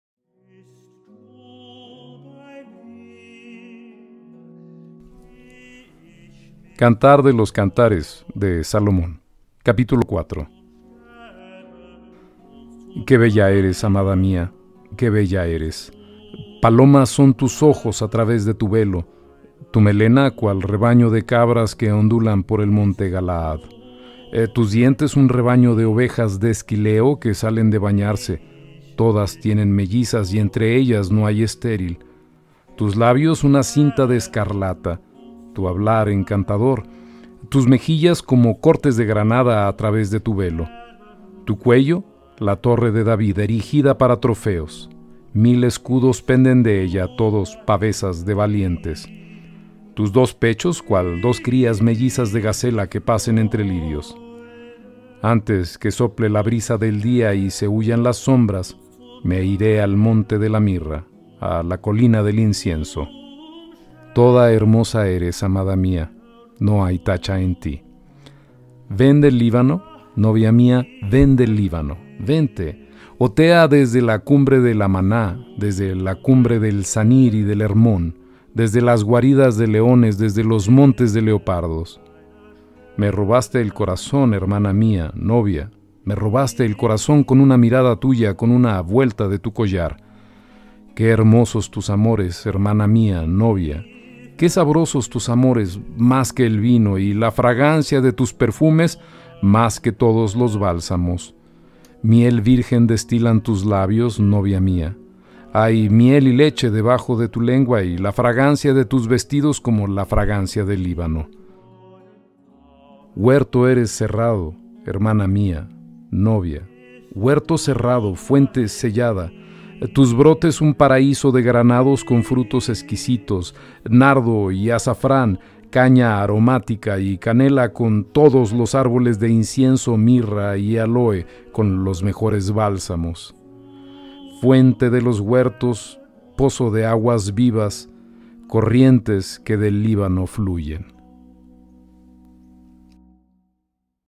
Cantar de los Cantares
Conmemorando el 35 aniversario de bodas con Ma. de Lourdes Mendoza Mendoza